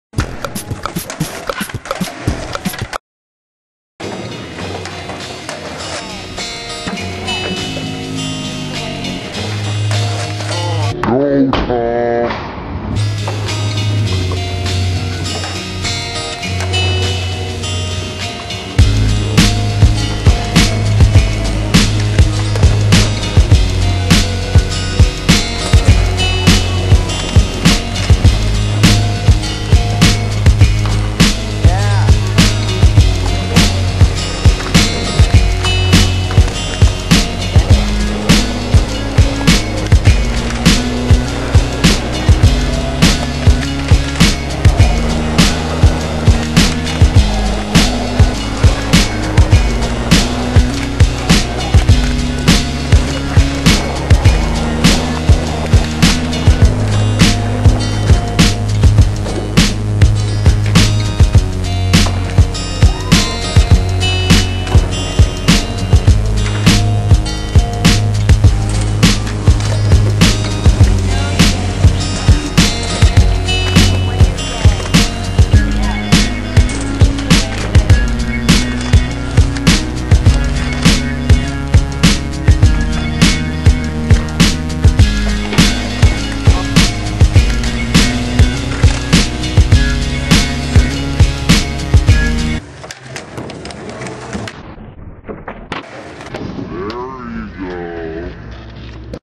Black Mamba Skatepark
a 2 day edit of dan pilewski at black mamba skatepark. last trick took forever. filmed by joey albanese